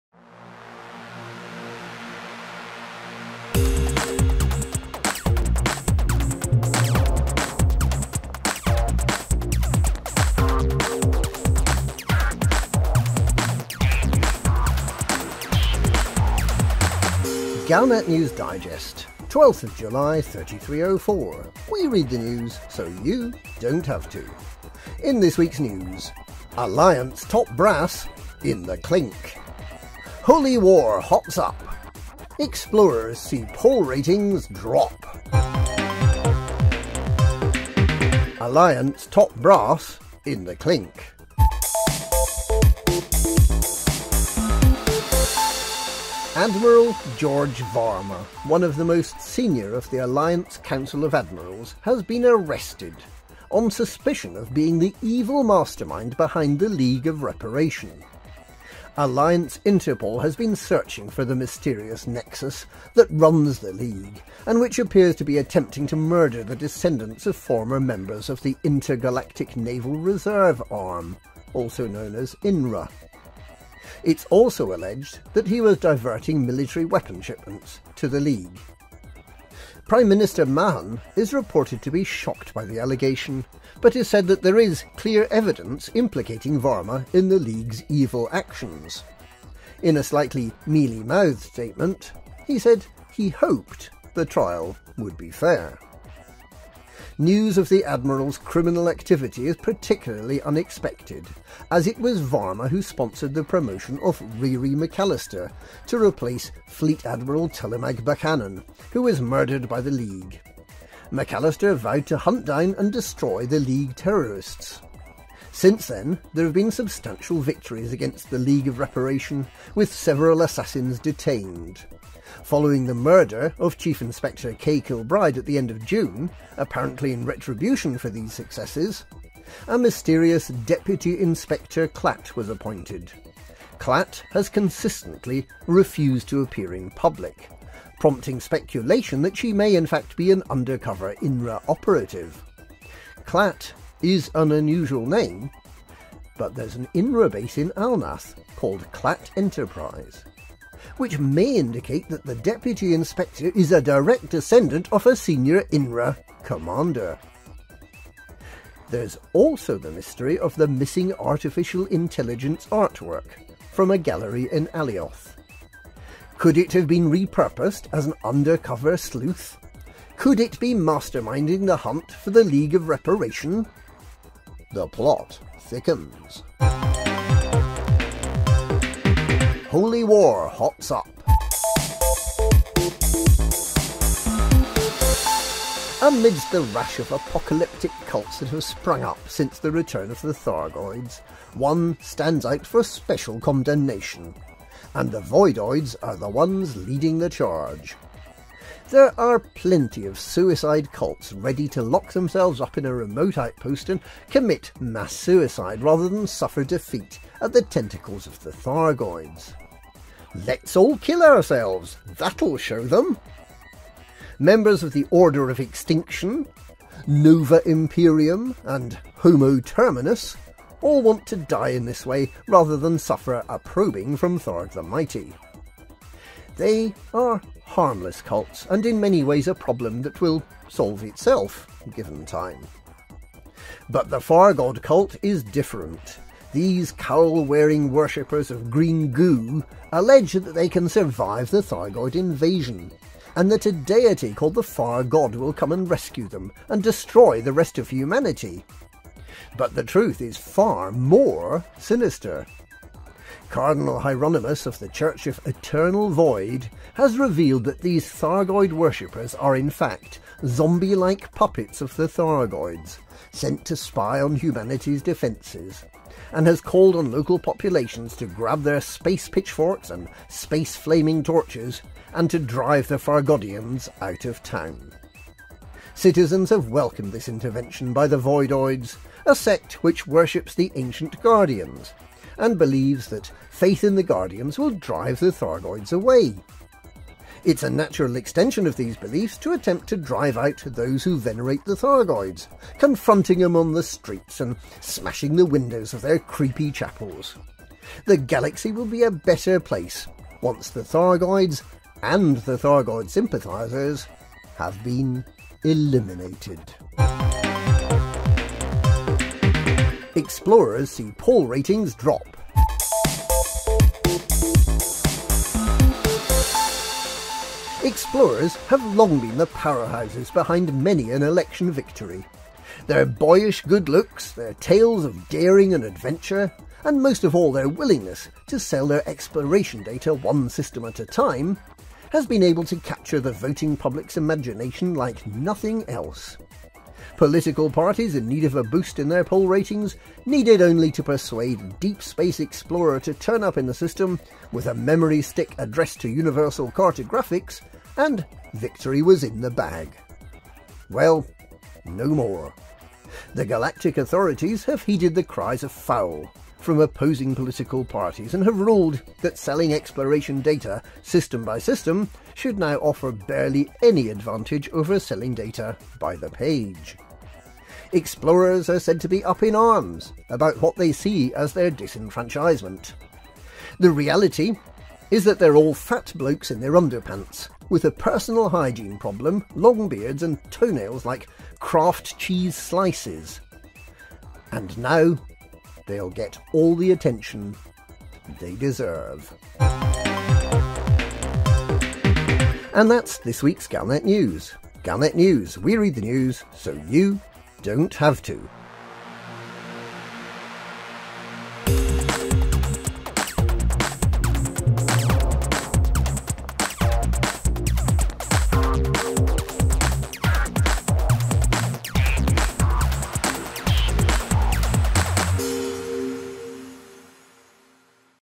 Galnet News Digest, 12th July 3304
We read the news so you don't have to. In this week's news:  Alliance Top Brass In the Clink. Holy War Hots Up. Explorers See Poll Ratings Drop.